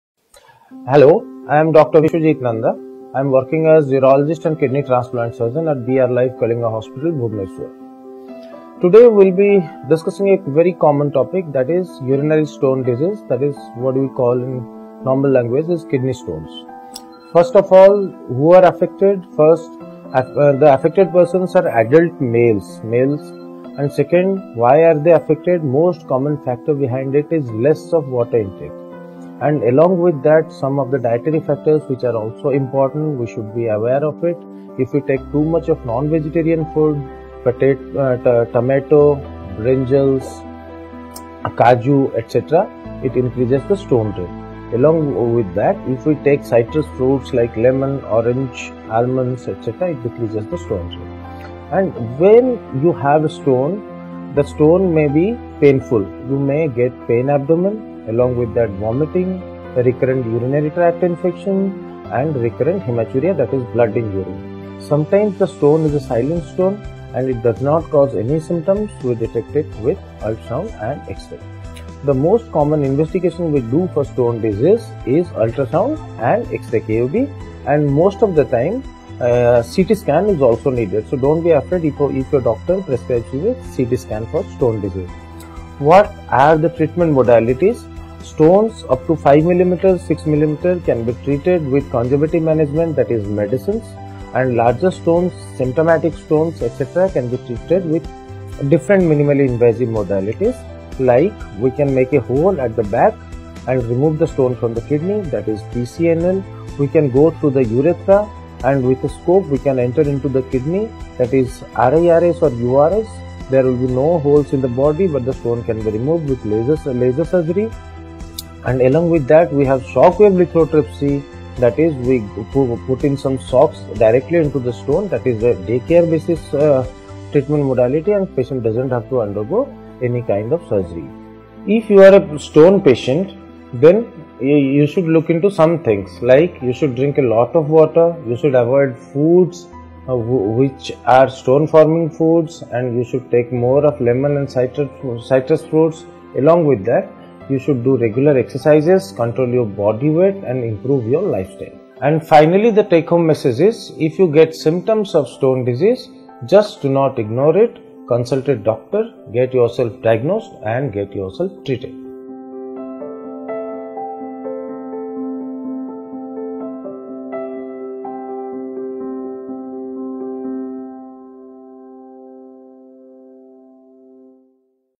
Kidney Stones and the treatment
In India, kidney stones affect close to 12% of the population - 50% of whom potentially end up with loss of kidney functions.

Watch our expert Dr. Biswajit Nanda talk about the causes, prevention and treatment for the same.